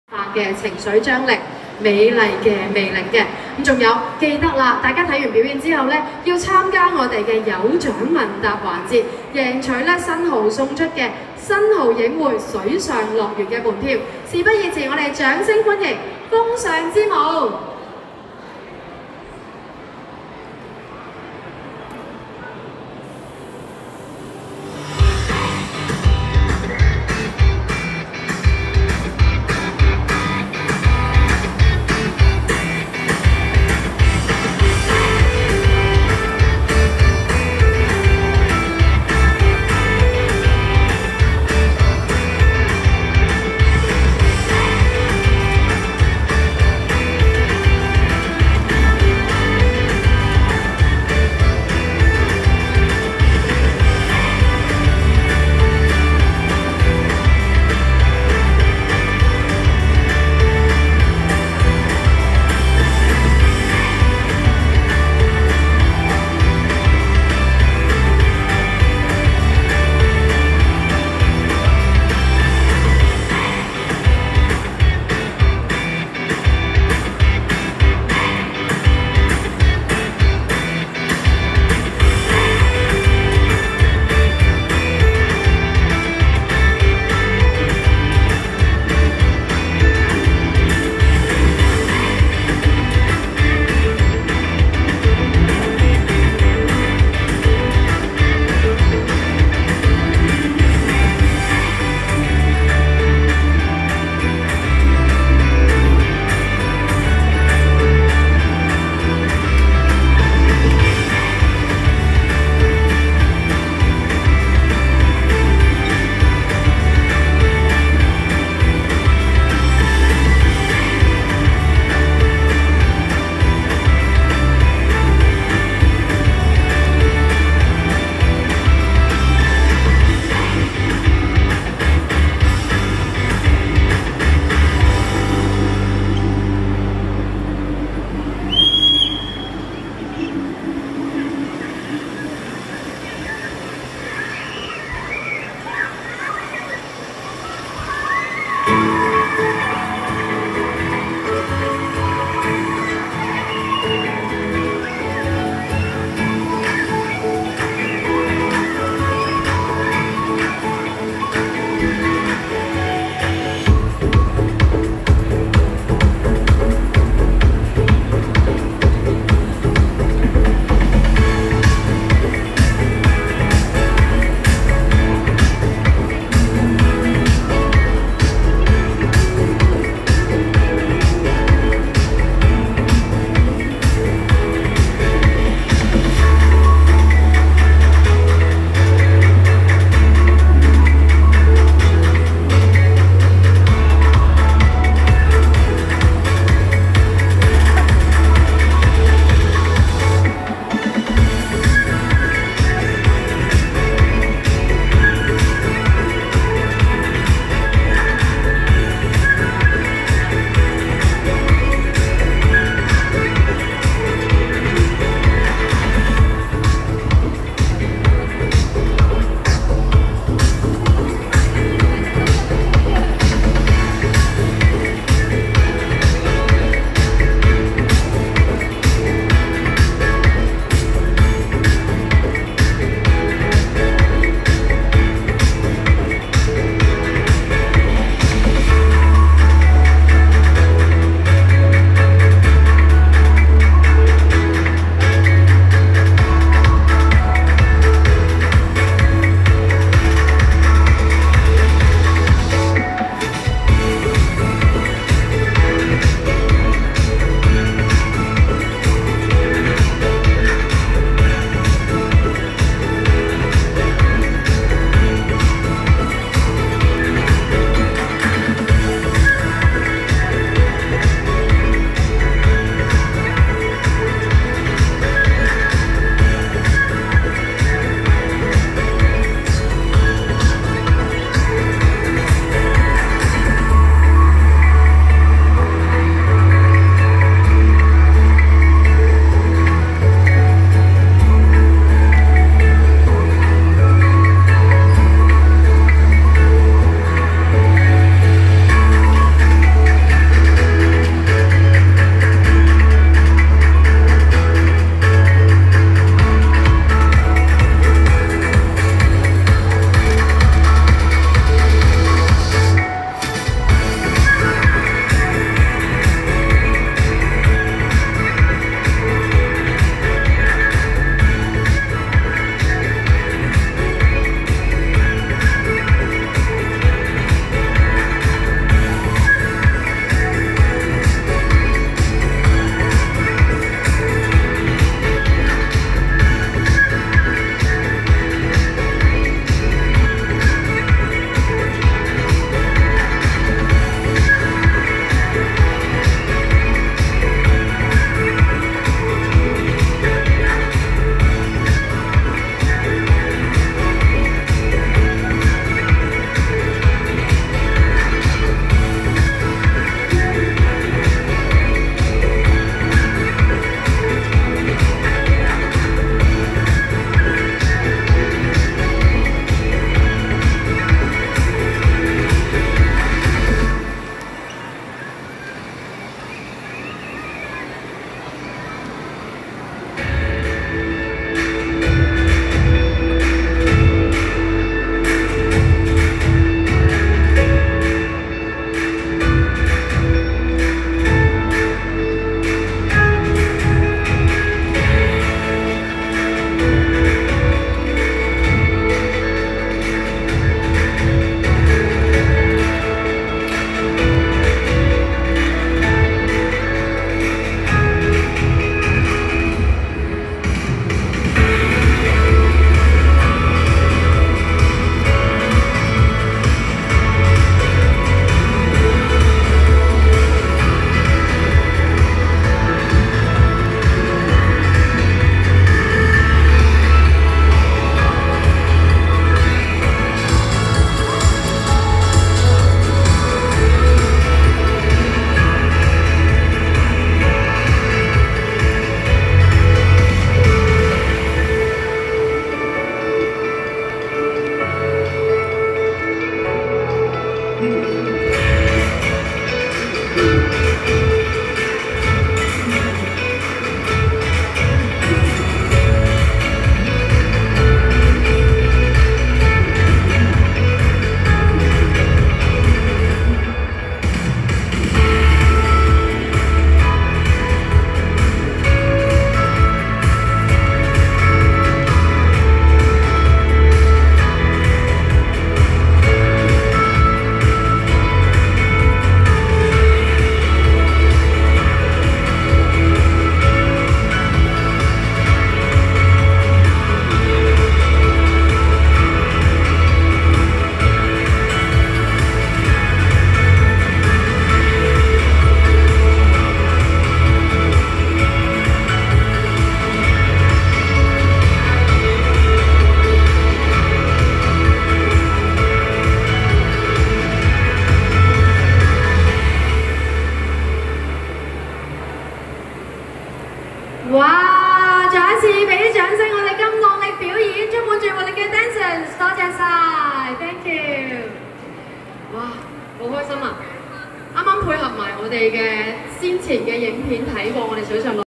natalie892領軍_風尚之舞 | 新濠博亞娛樂【感受澳門嘉年華 精彩活動展繽紛】 | 感受澳門Experience Macau | 20240518 163310
「感受澳門嘉年華」路展已於5月17-19日一連三日喺東港城一樓中庭舉行🥳。嘉年華期間，除咗有豐富多彩嘅節目之外，仲有多項獎品同優惠🤩，為市民帶嚟一場獨特嘅澳門特色盛夏之旅💞。

😆 精彩台上表演及有奬問答遊戲環節（5月18－19日；時間：下午1時至7時）
六間「澳門綜合旅遊休閒酒店」帶來了耳目一新的連場精彩表演，包括跳舞、唱歌、武術等等🤪！

🌟「感受澳門」嘉年華
日期：2024年5月17日至5月19日
時間：中午12時至晚上8時
地點：東港城 East Point City一樓中庭